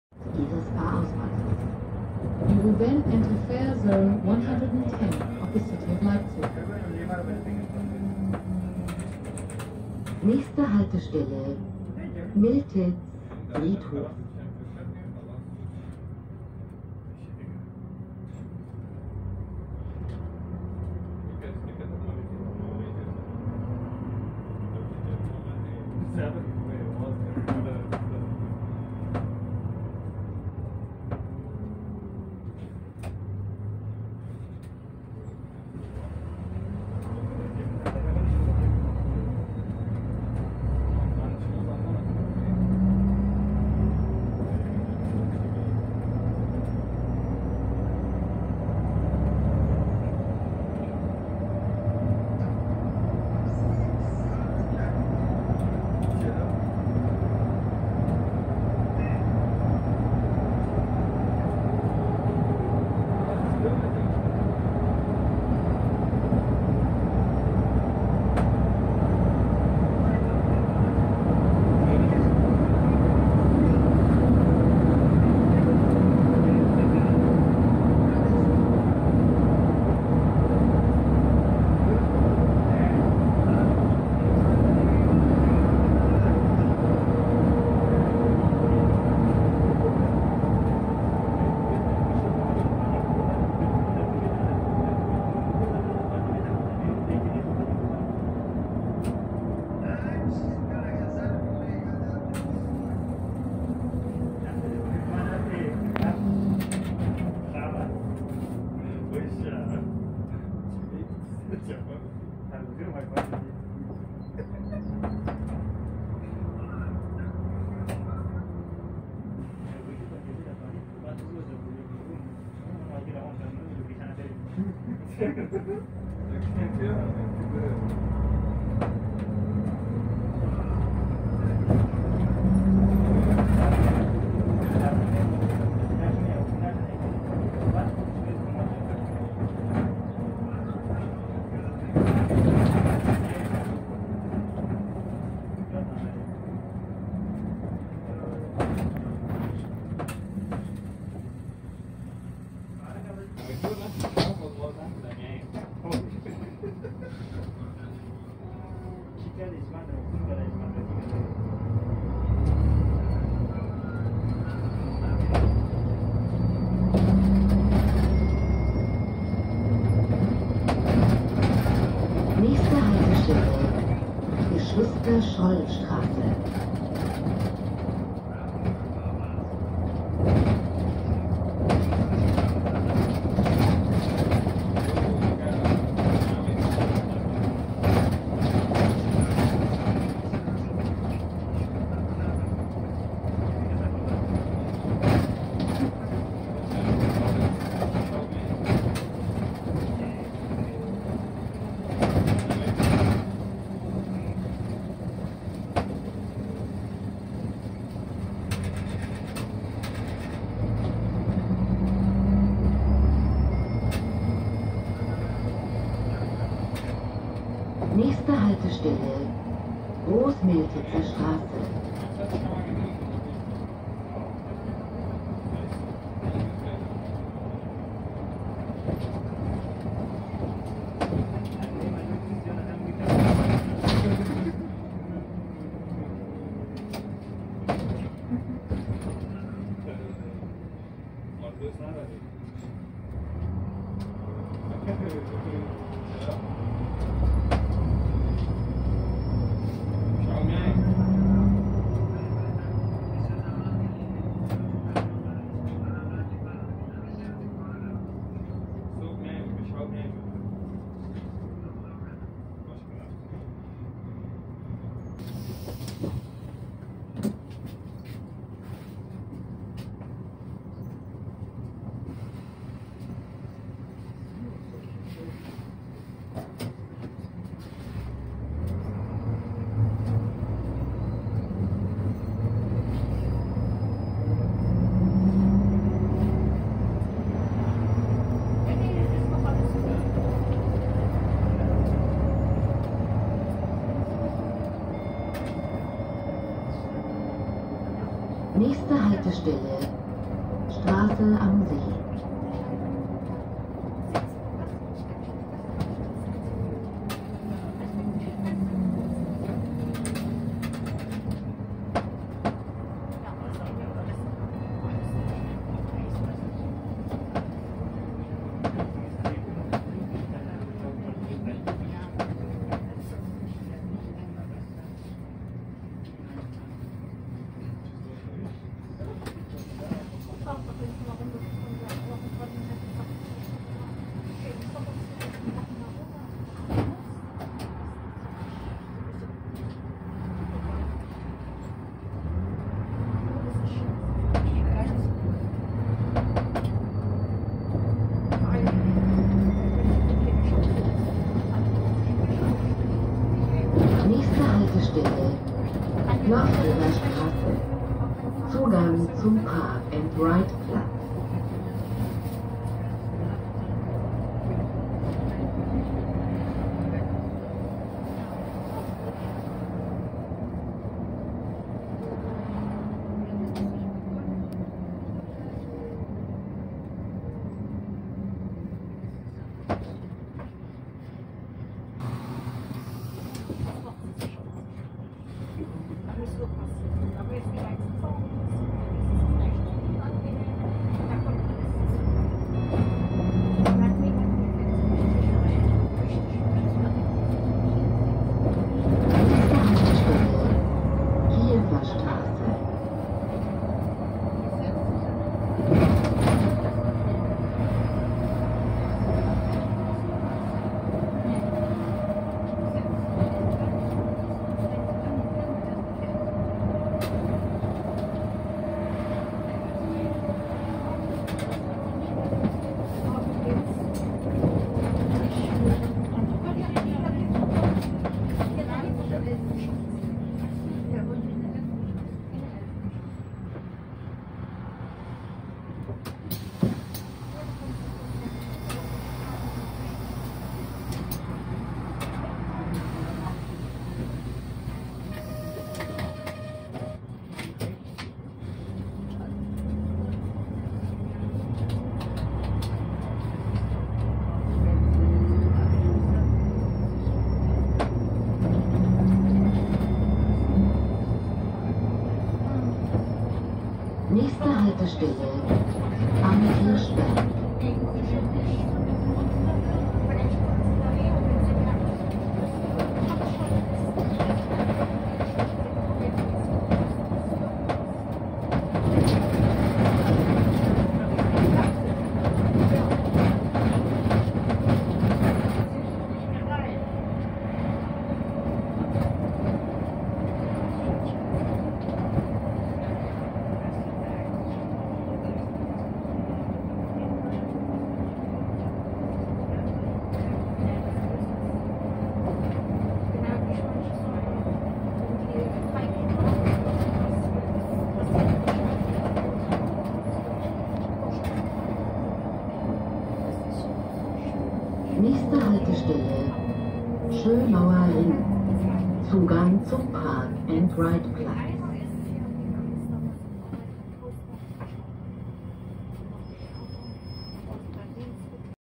Linienfahrt 65 von Markranstädt Bhf nach Leipzig Schönauer Ring mit MAN NÜ 280
Details:
VB: LVB
Matrix: 65 Markkleeberg
WN:12167
Kennz: L-VR 5044